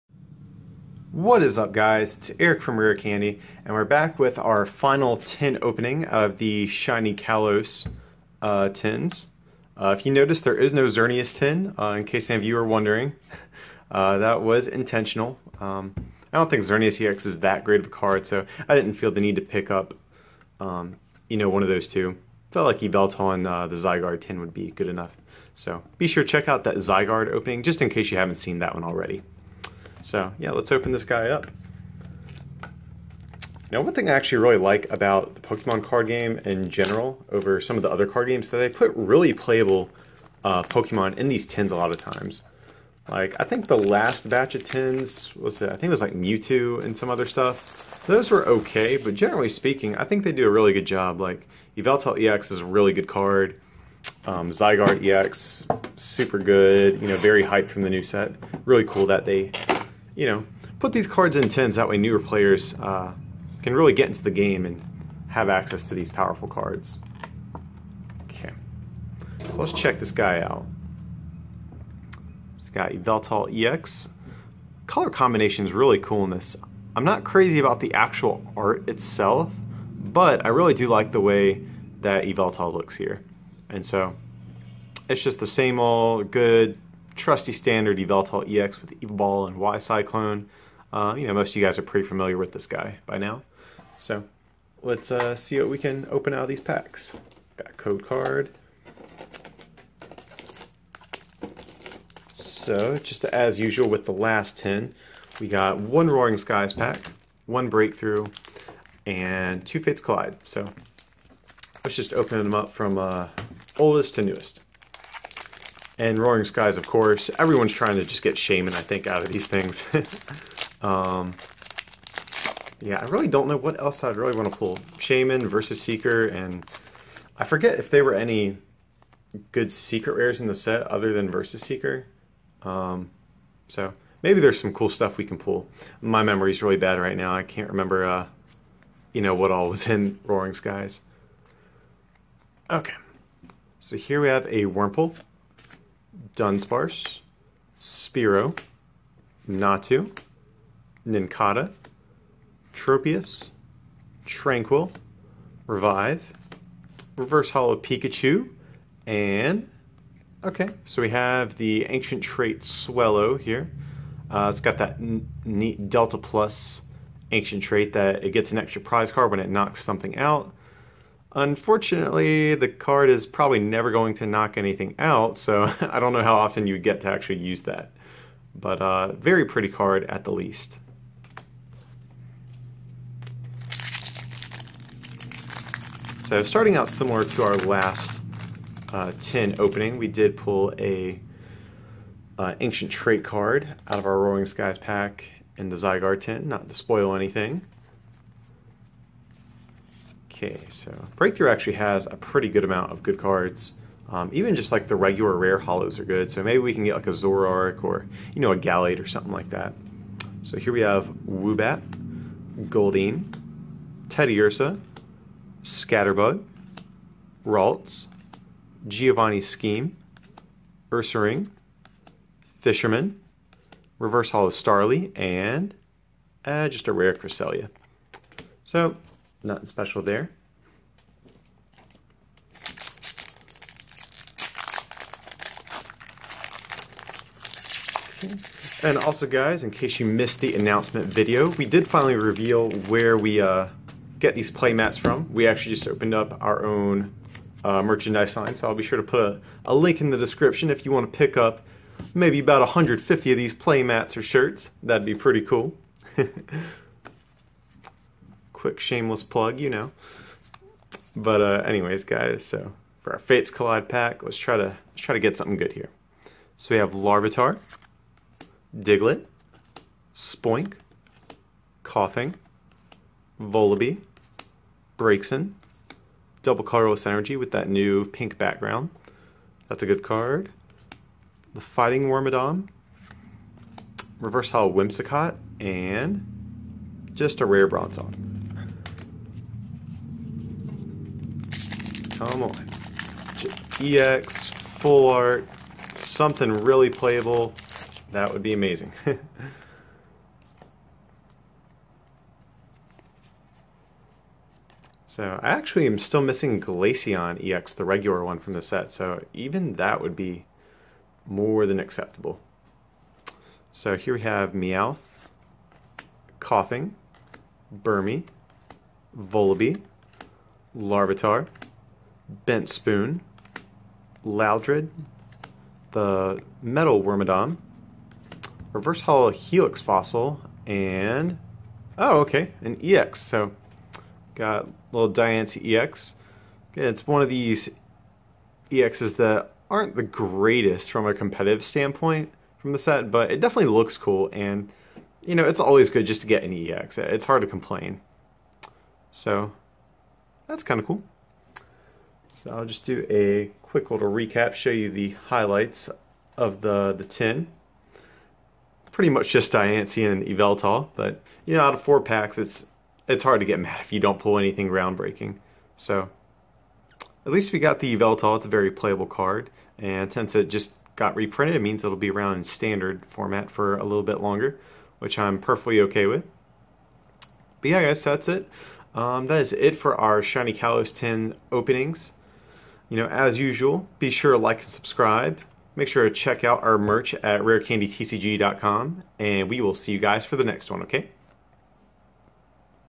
Pokemon Yveltal EX Shiny Kalos Tin Opening (EX PULL)
We get our hands on some of the new Shiny Kalos Tins and try our luck at opening some more booster packs from the newest Pokemon XY expansion Fates Collide. For this one we open up one of the Yveltal EX tins. Will we pull anything good?